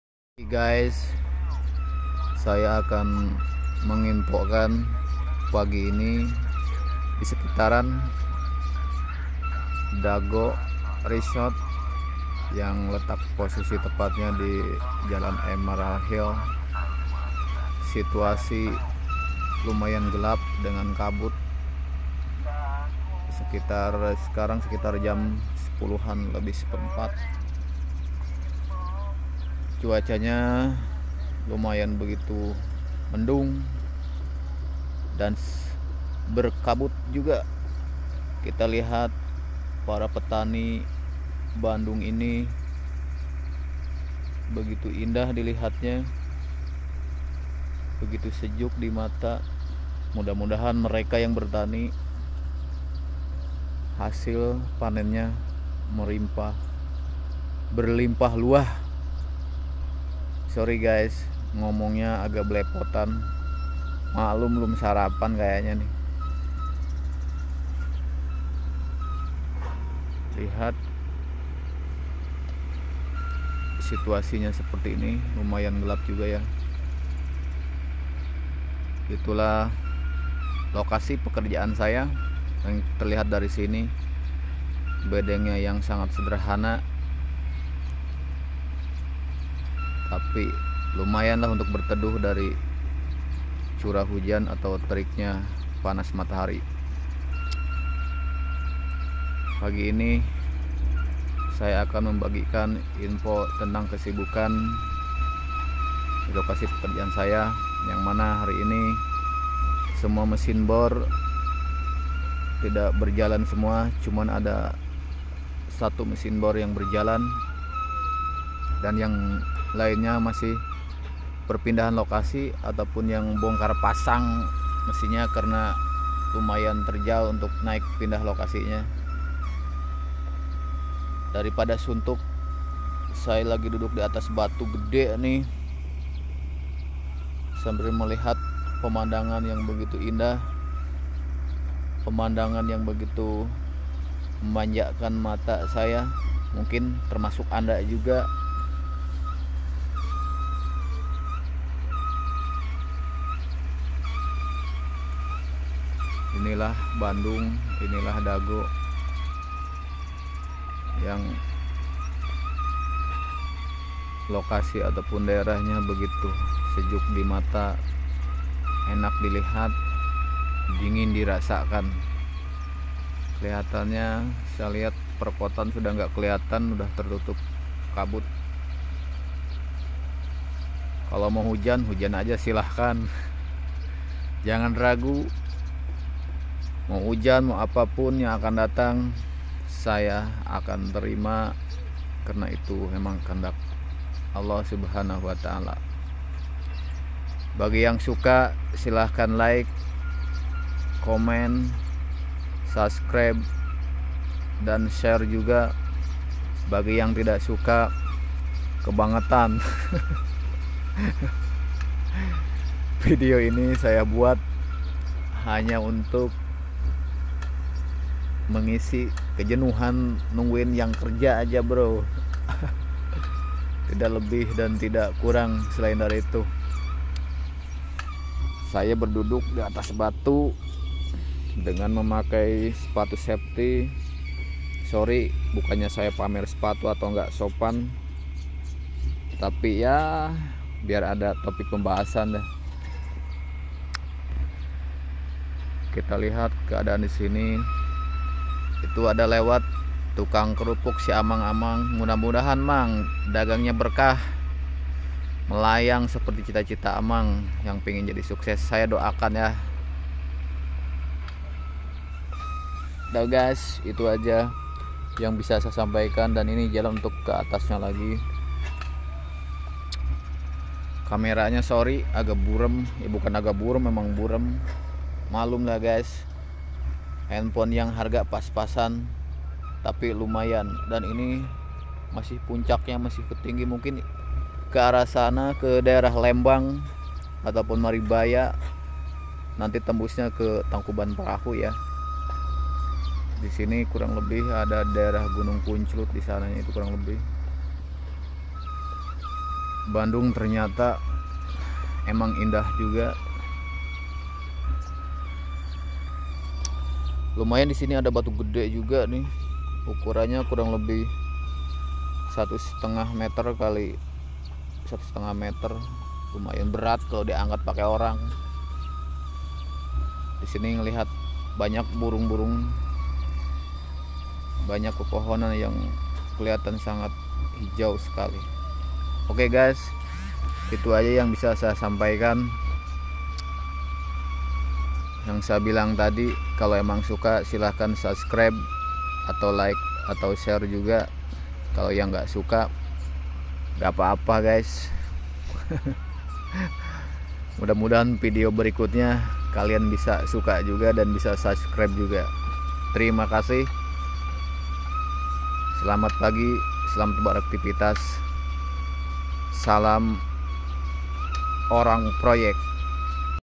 MANJAKAN MATA DAN PIKIRAN...
ASSALAMUALAKUM...PAGI SOBAT ...ITULAH KONDISI SAAT ITU KABUT LUMAYAN TEBAL DI PERBUKITAN DAGO RESORT BANDUNG.